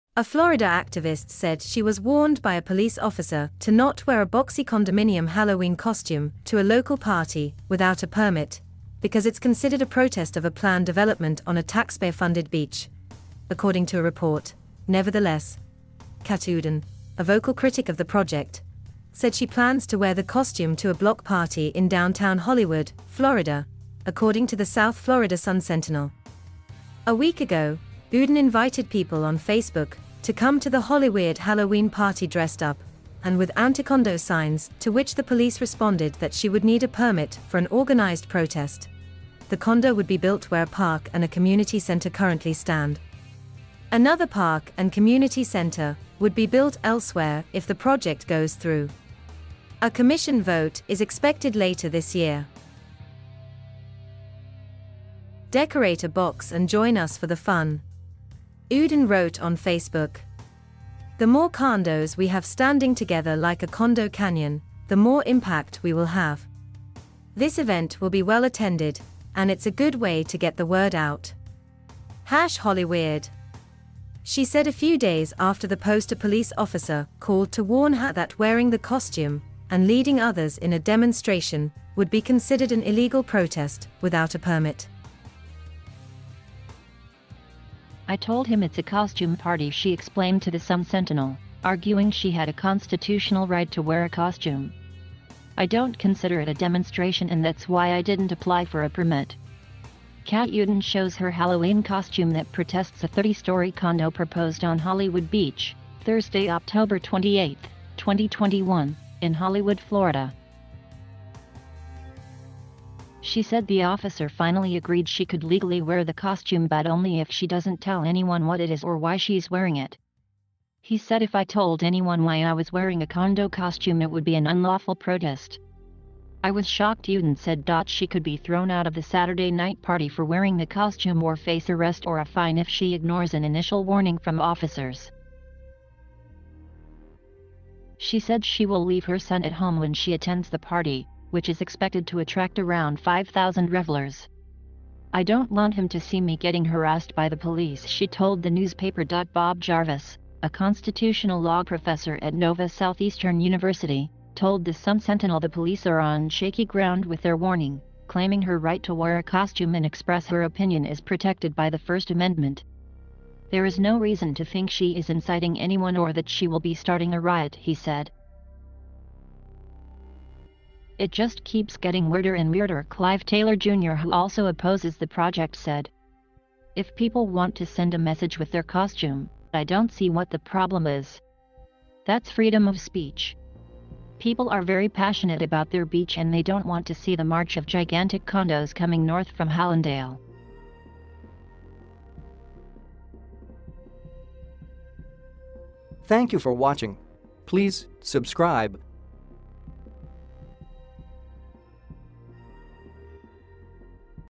Florida woman says police told her not to wear her Halloween costume without a permit
News of the world: Florida woman says police told her not to wear her Halloween costume without a permit

A Florida activist said she was warned by a police officer to not wear a boxy condominium Halloween costume to a local party without a permit because it’s considered a protest of a planned development on a taxpayer-funded beach, according to a report.Nevertheless, Cat Uden, a vocal critic of the project, said she plans to wear the costume to a block party in downtown Hollywood, Florida, according to the South Florida Sun-Sentinel.A week ago, Uden invited people on Facebook to come to the Hollywe

death-by-desire. alien-invasion.